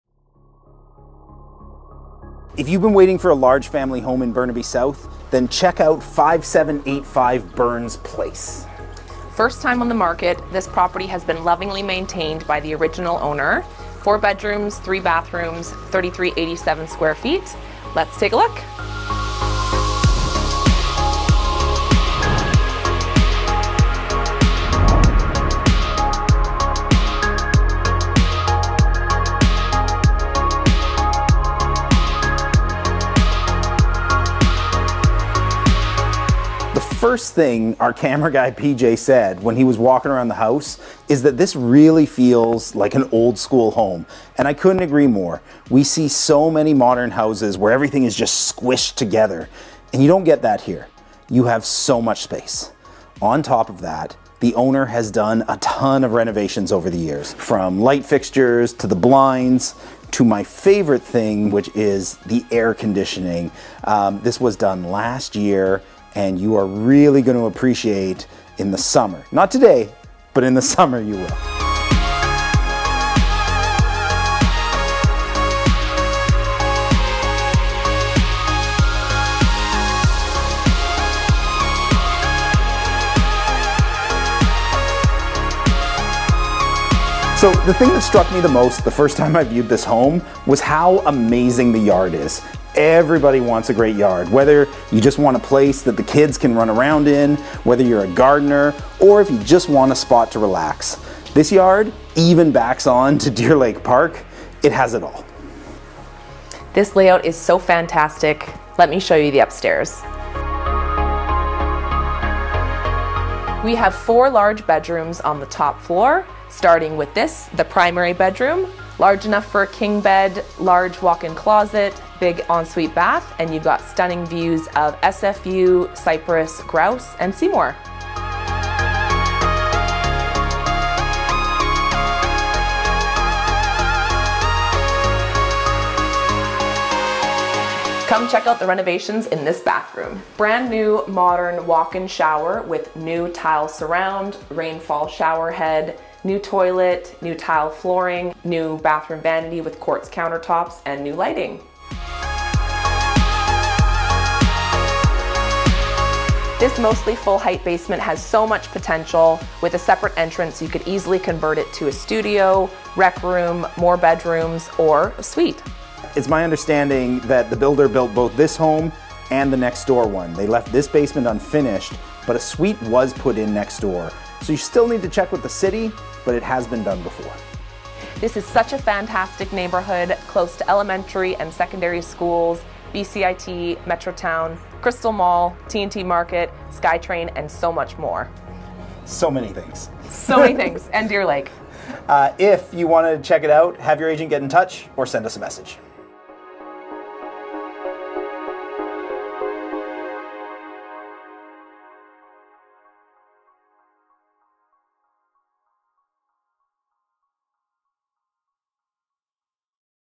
Deer Lake House Sold In Multiple Offers - 5787 Burns Place, Burnaby
Welcome to your dream home! This 4-bedroom, 3 bathroom residence has been lovingly cared for by its original owner and is ready to make new memories with your family. Nestled in a highly sought-after Upper Deer Lake, this property offers a perfect blend of comfort, convenience, and potential. All four bedrooms are conveniently located on the upper level, offering both privacy and proximity for your family. Updated include a fully renovated bathroom in 2016, air conditioning installed last year, laminate floors, double pane windows, furnace replaced in 2009, roof replaced appx 10 years ago. Home includes a 2 gar garage and an unfished basement ready for your ideas with its own entrance. The beautiful yard back onto Deer Lake Park. Don't miss this chance to make this house your home!

$2,199,900
4 Bed
3 Bath
3,387 sq ft